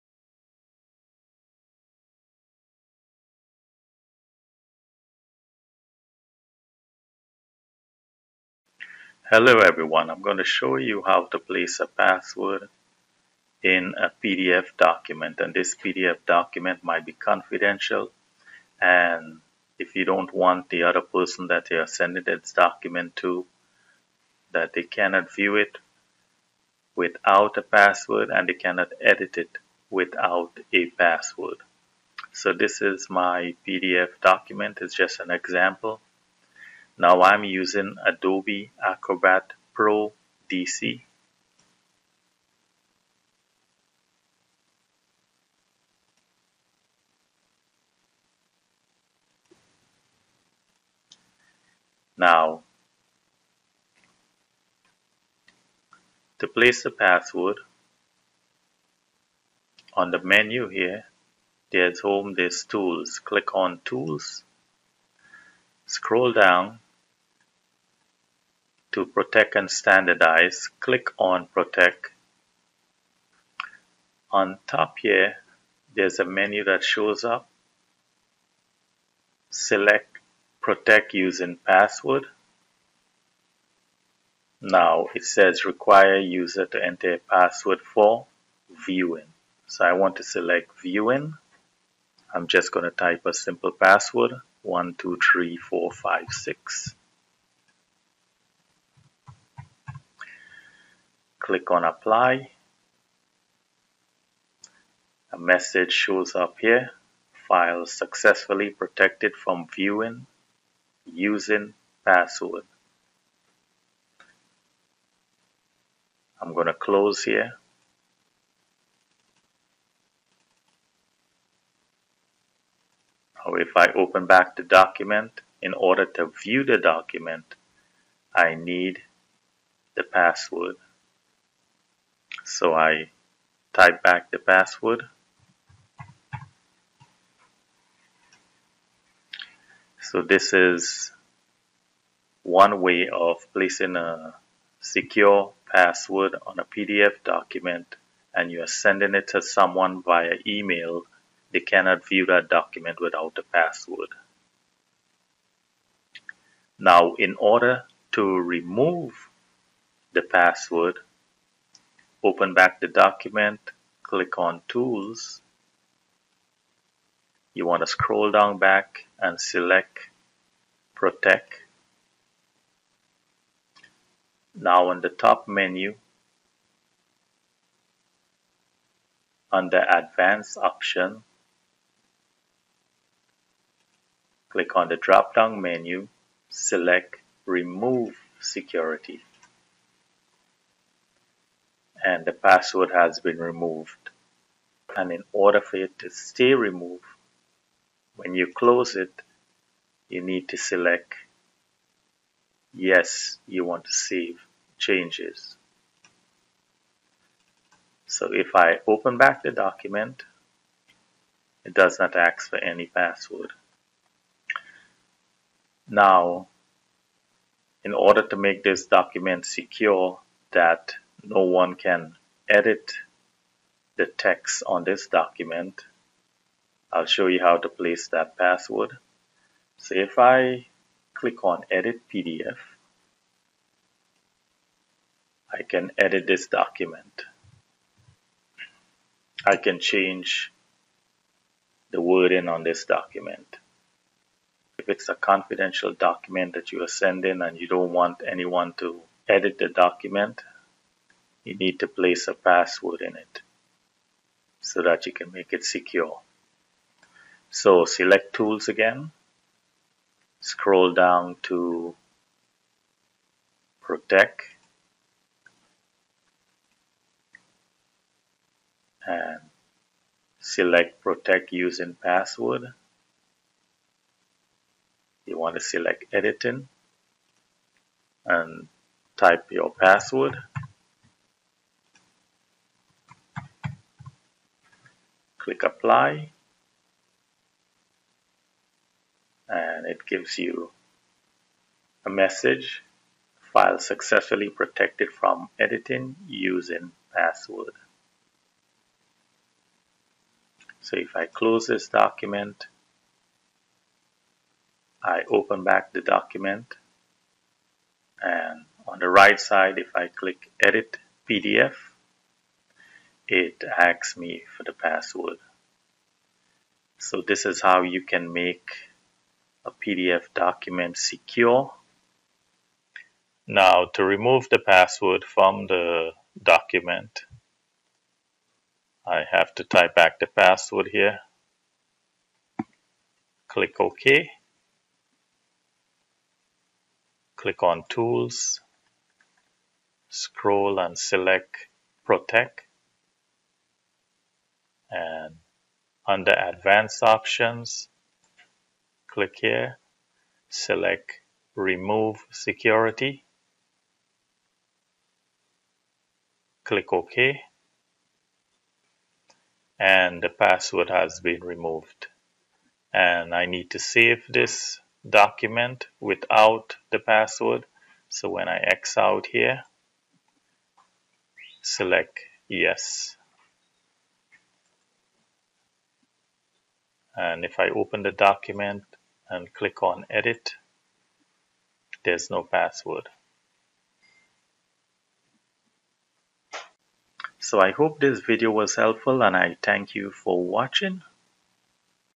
How To Password Protect A PDF Document
How to place a password in a pdf document. Make your document secure with a password so that no one can view or edit the pdf without the required password.